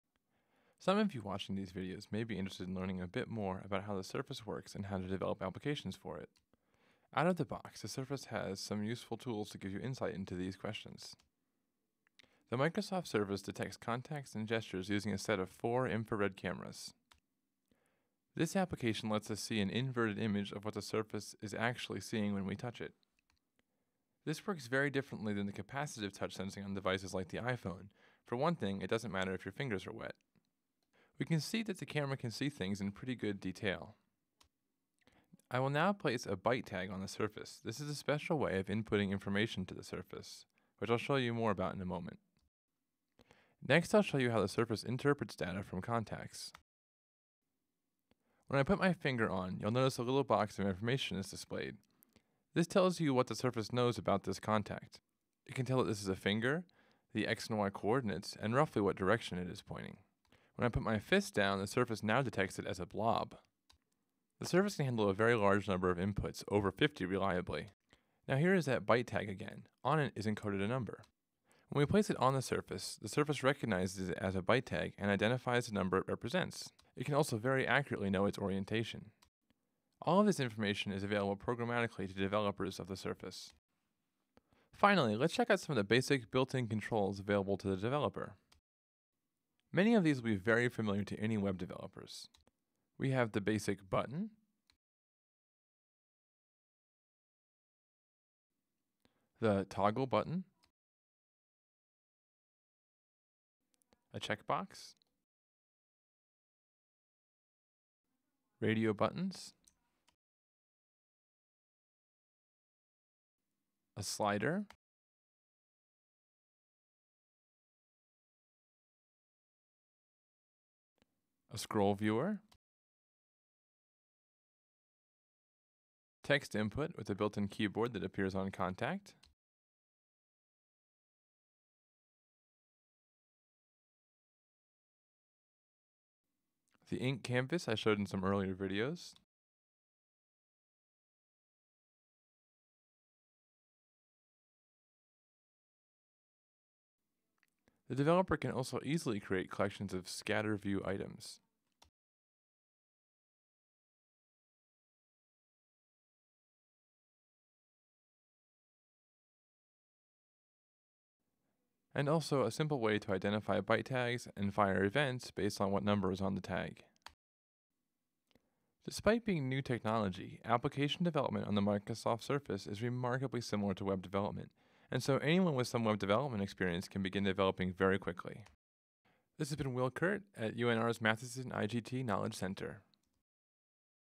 Development on the Microsoft Surface
An overview of how the Surface works and understanding how development works on the Microsoft Surface